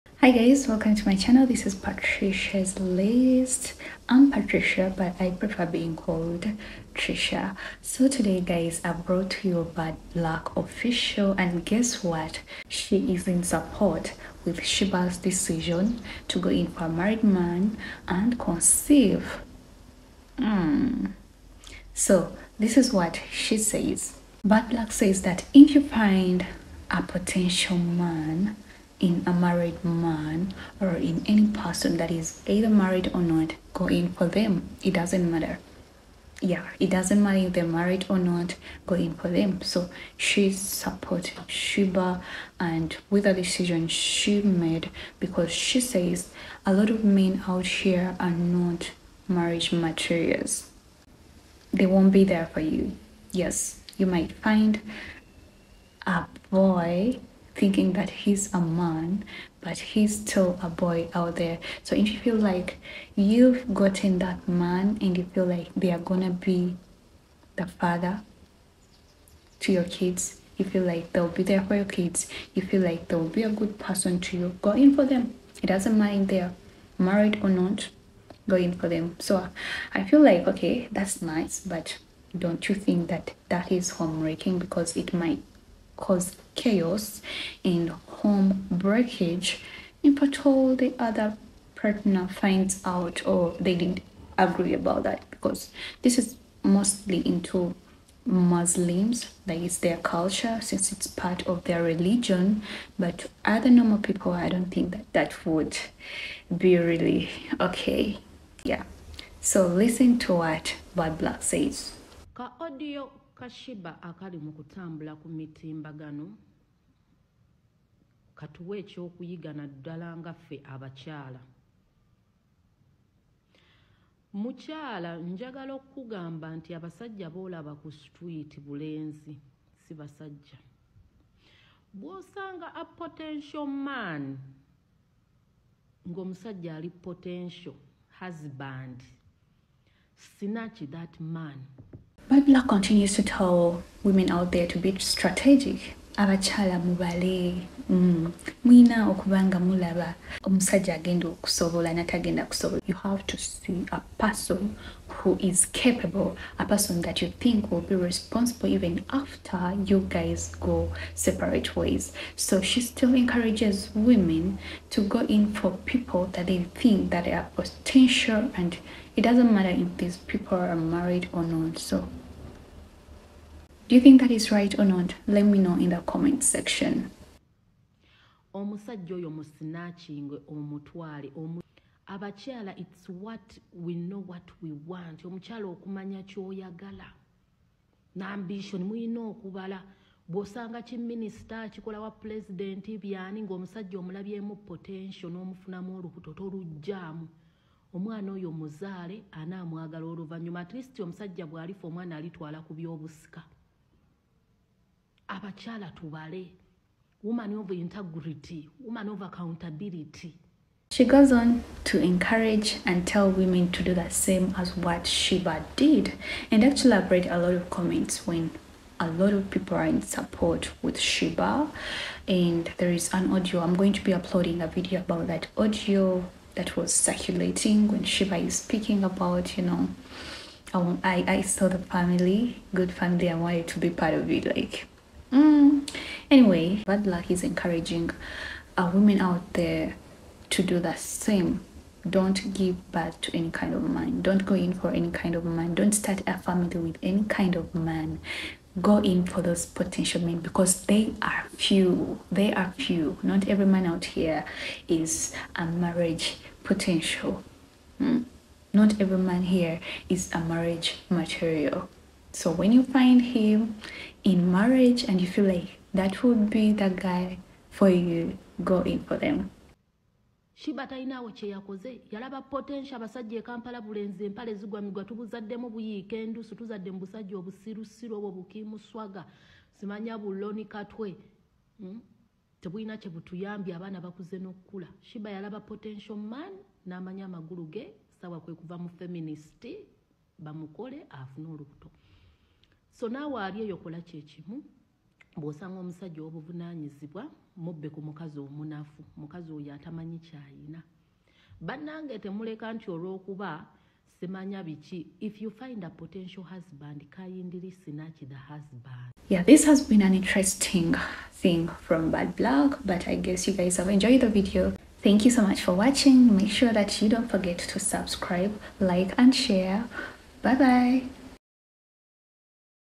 Bad Black Defends Sheebah Karungi Over Alleged Affair with Married Man | Shocking Support Explained!
Ugandan socialite Bad Black has caused a stir after publicly supporting Sheebah Karungi amidst allegations of being involved with a married man. In this video, we dive into Bad Black’s shocking statements, why she’s standing by Sheebah, and how fans are reacting to this unexpected alliance.

Is this support justified, or is it fueling controversy? Watch to find out more about this trending drama in Uganda’s entertainment scene.

What You’ll See in This Video:

Bad Black’s bold statements supporting Sheebah

Public reactions to the alleged affair

Analysis of Sheebah’s response to the rumors